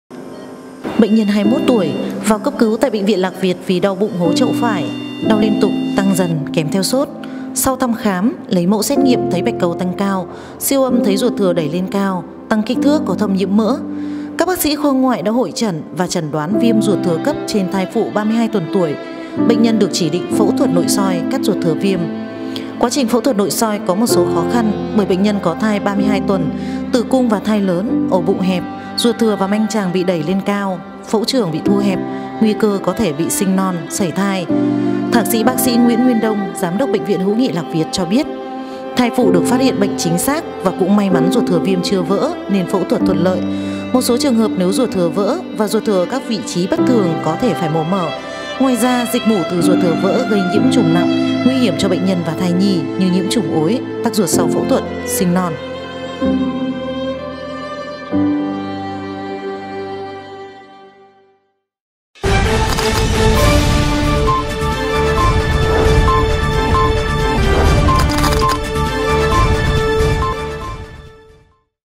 PHẪU THUẬT THÀNH CÔNG CA VIÊM RUỘT THỪA CHO THAI PHỤ CÓ THAI 32 TUẦN | Bệnh viện Hữu Nghị Lạc Việt.
🏥 Bệnh viện Hữu Nghị Lạc Việt
📎 Nguyễn Tất Thành, Định Trung, Vĩnh Yên, Vĩnh Phúc

© Bản quyền thuộc về Bệnh viện Hữu Nghị Lạc Việt.